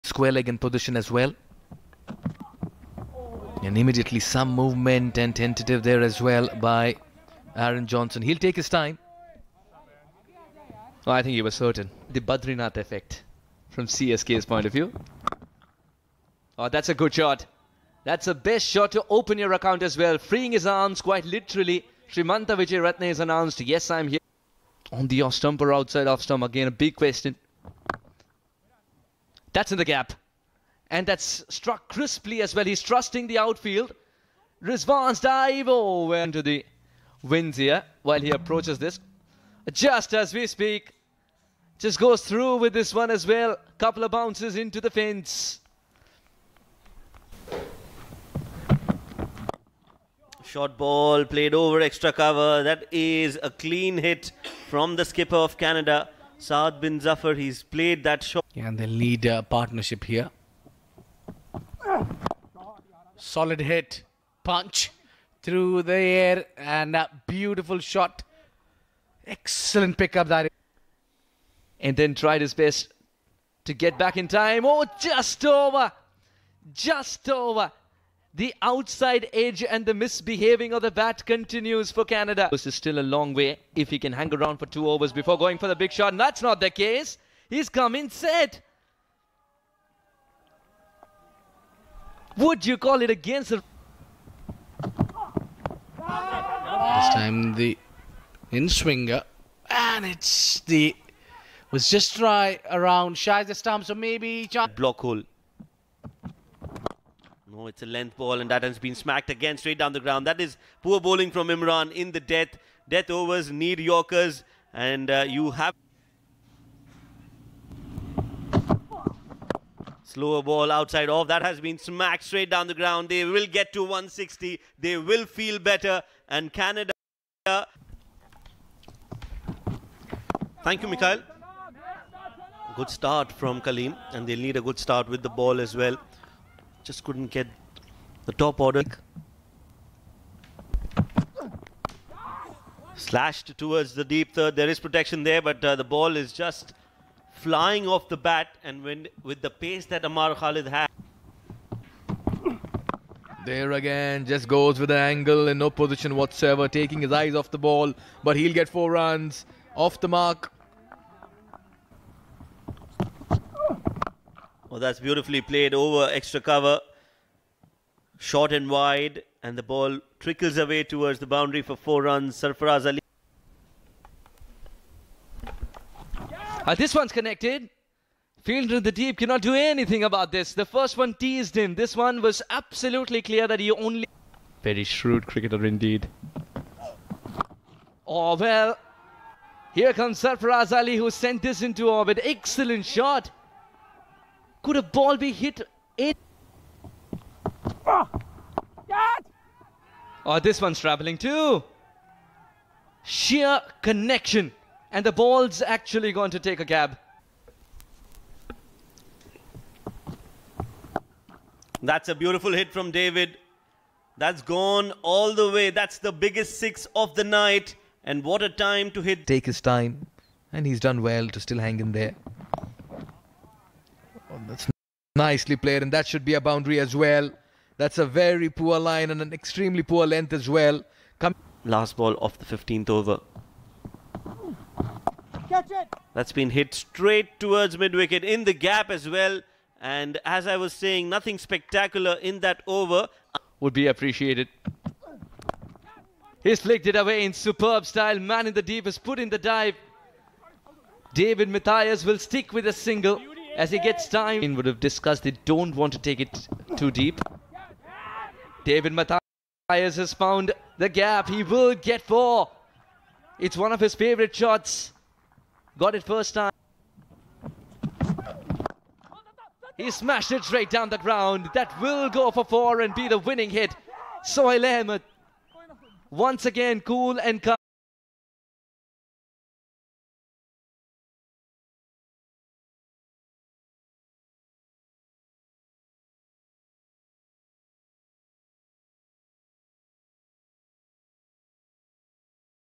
Highlights: 8th Match, Canada vs Bahrain
🏏: Oman Desert Quadrangle T20 Series 2022
📺: Willow TV